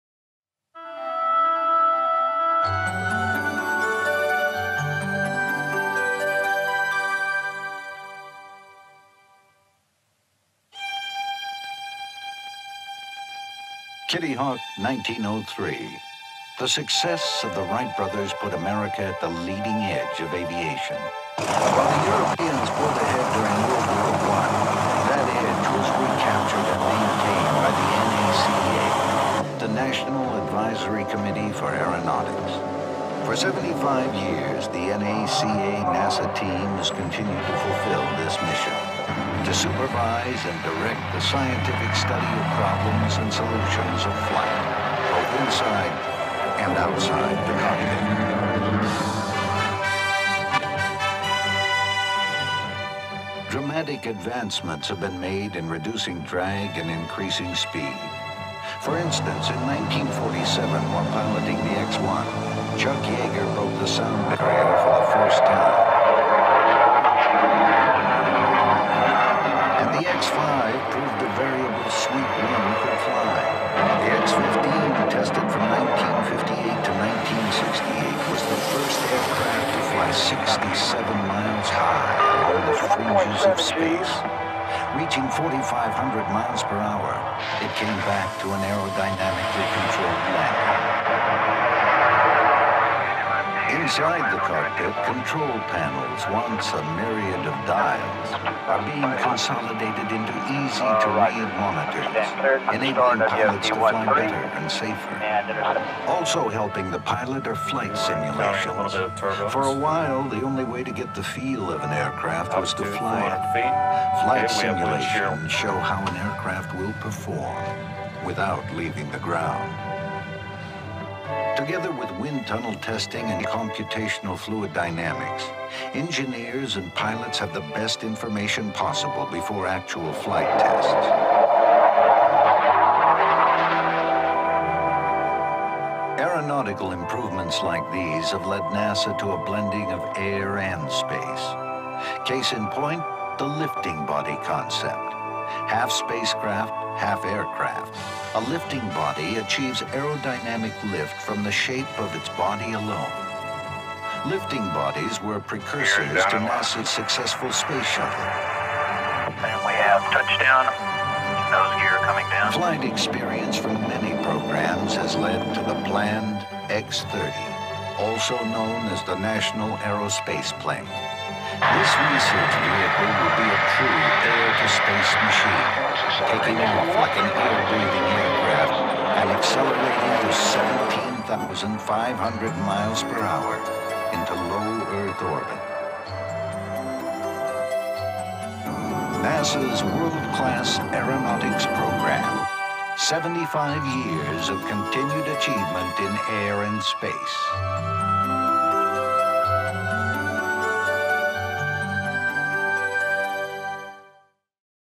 NACA-NASA: 75 Years of Flight - CharlieDeanArchives / Archival Footage
This document presents historical footage used to recollect the last 75 years of aeronautical and space-related research.

This film made available courtesy the Department of Defense, National Technical Information Service, and the National Archives and Records Administration